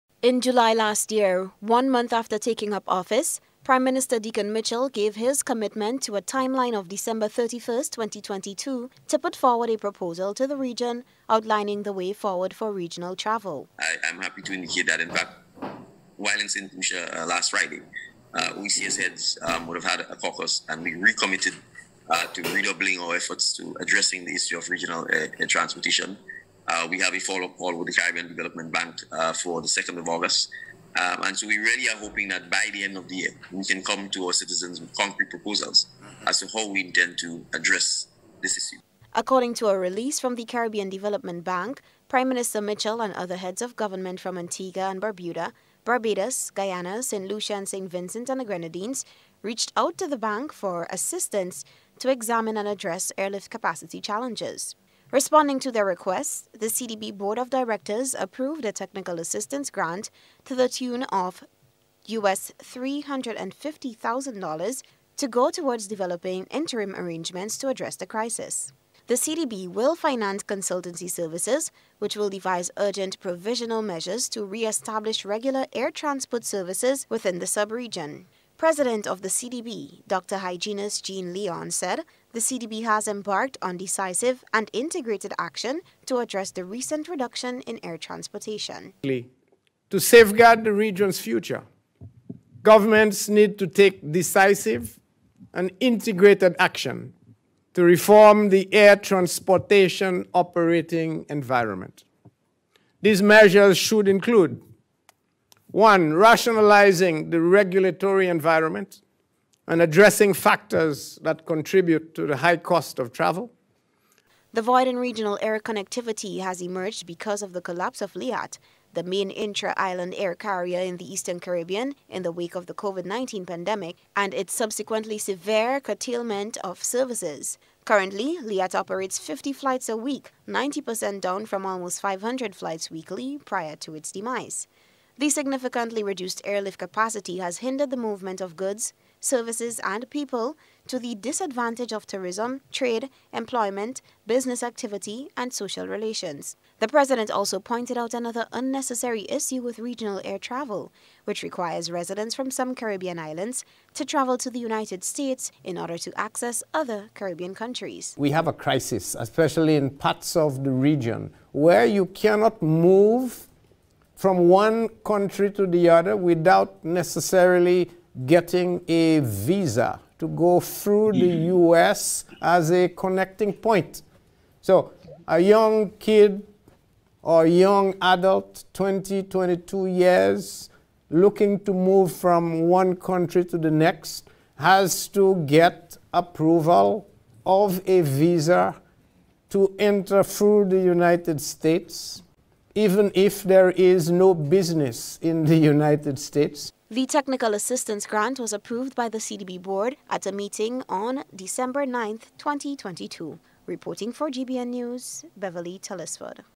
A Fund To Address Regional Air Transportation
Responding to the requests of prime minister Dickon Mitchell and other Caribbean leaders for an amicable solution to the concerning decline in regional air travel, the Caribbean development bank announced it has funded a consultancy to address the Eastern Caribbean Air Transport crisis.